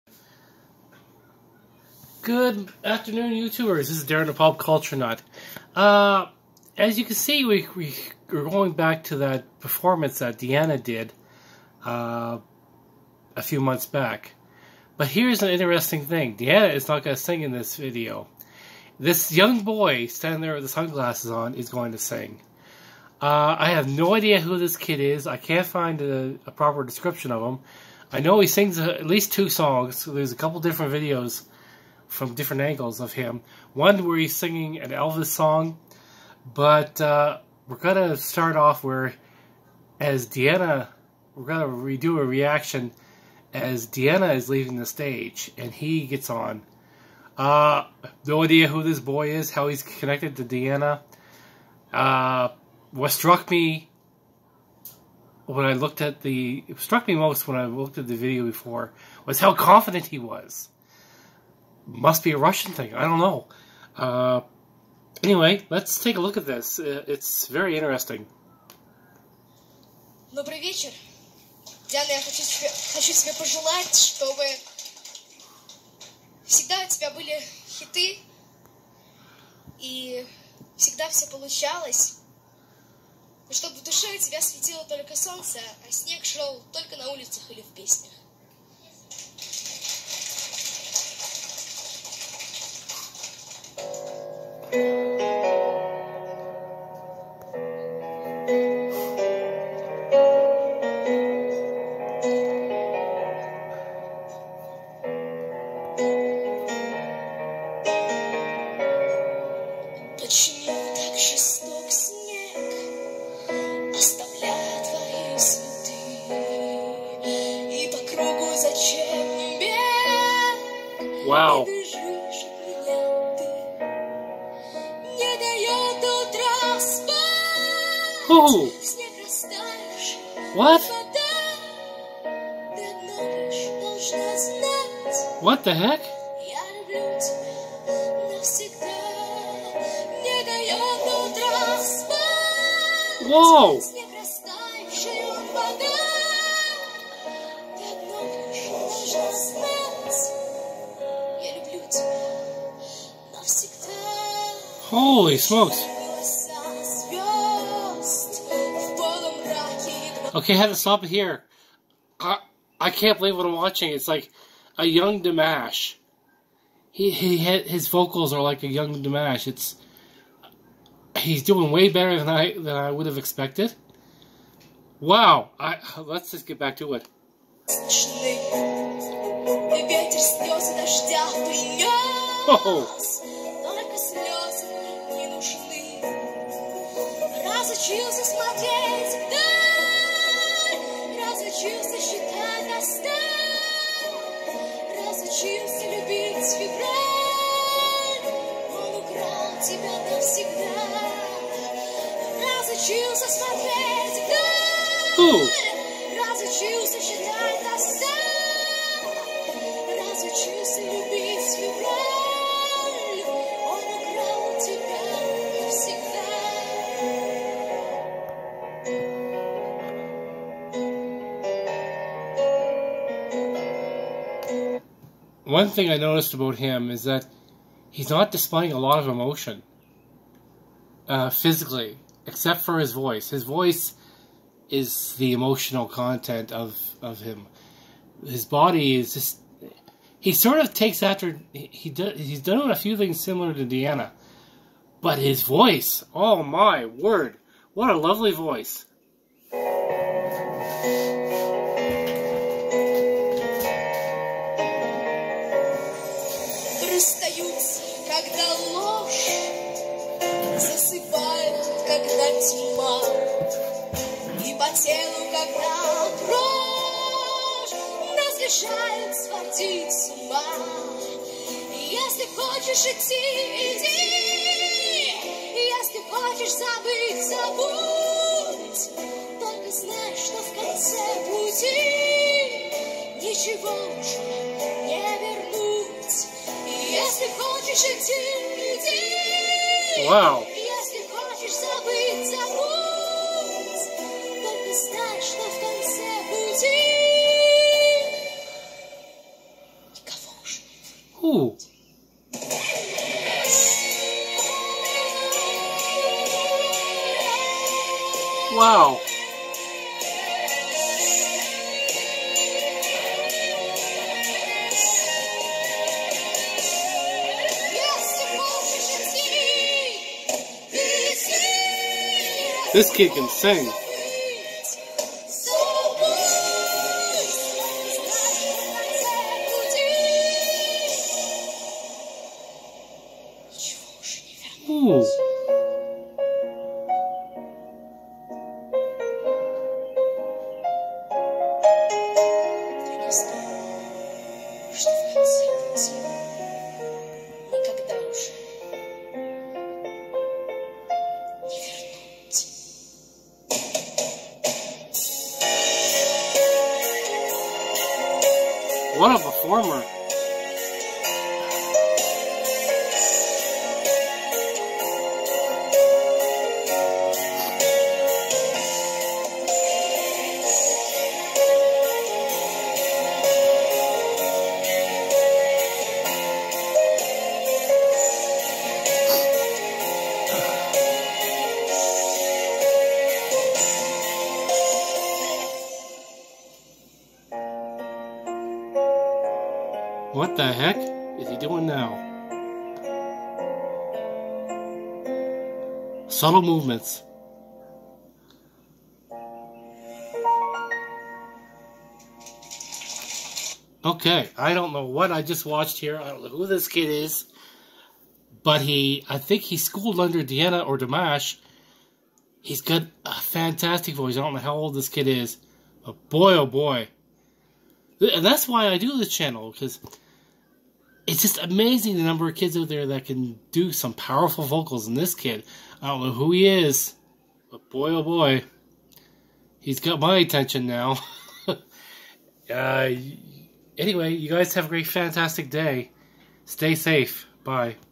OMG! WHO IS THIS KID? possibly part 1!
This kid has a fantastic voice and sang after Diana! To see more, visit Диана Анкудинова on youtube. How is he connected to Diana? I have so many questions!